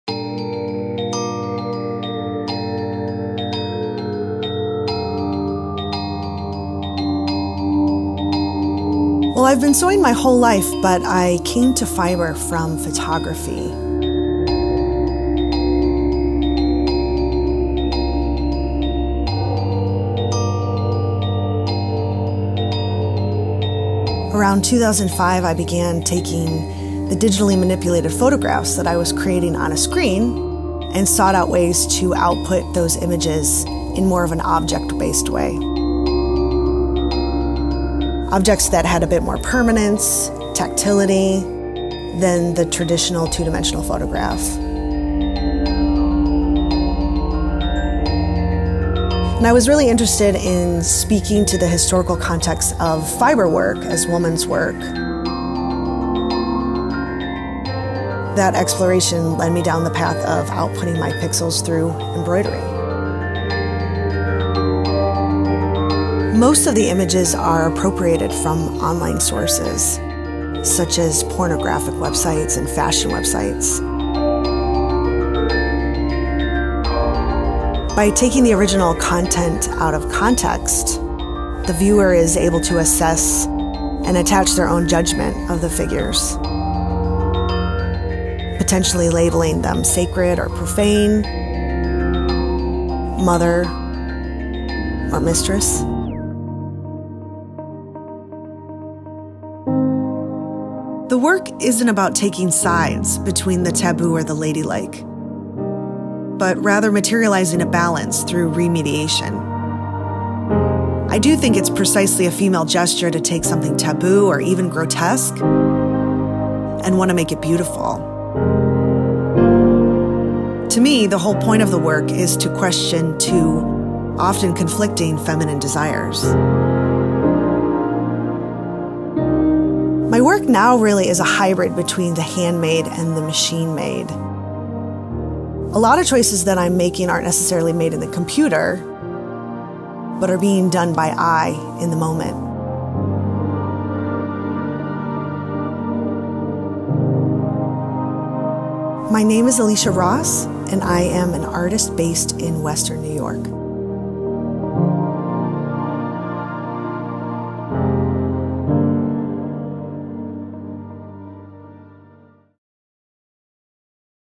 Alicia Ross, The Distress of Uncertainty
Artist Alicia Ross's artwork explores various aspects of female identity using photography, fiber, video, and installation. Most recently noted for her large-scale embroidery work, Ross’s work embodies a unique hybridization between hand and machine. Ross's work blurs lines between the sacred and profane—as subject matter is appropriated from online sources, removed from their original context, and translated into stitch. Through manipulation and remediation, Ross forces the viewer to ascribe their own moral virtue to the figures. Alicia Ross's work evokes dialog regarding the female form and the social construction of gender roles within the context of the male gaze.
Credits:
Camera, Audio: Alicia Ross